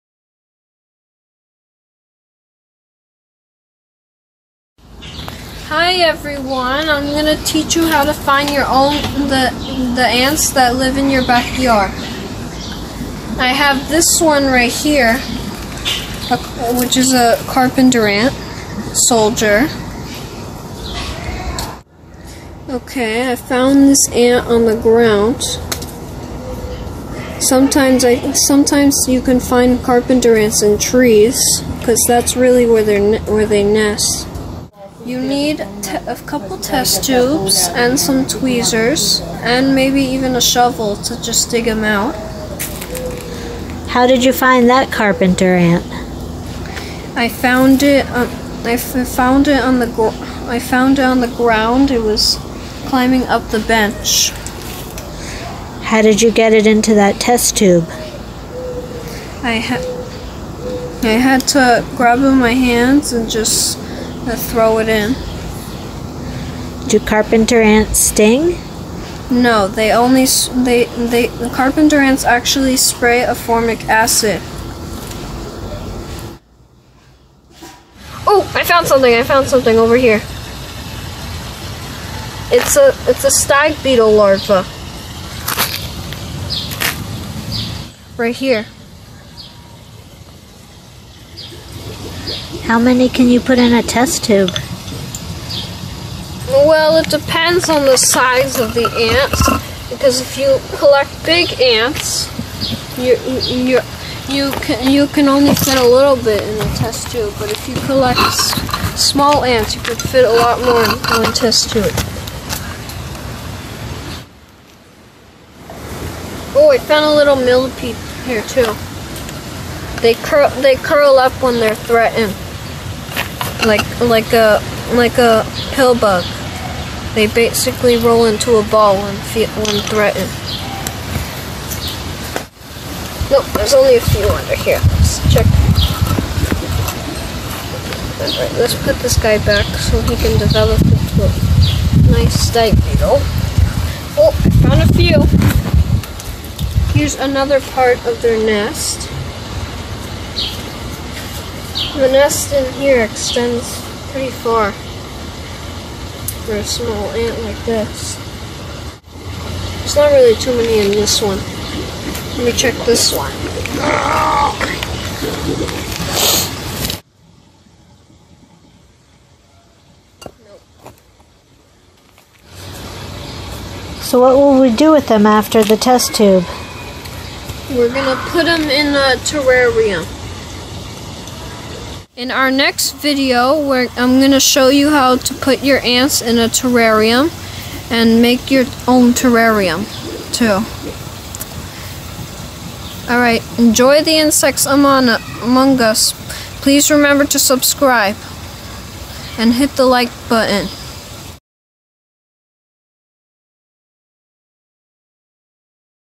Finding ants
Look at how I found carpenter and pavement ants in my urban backyard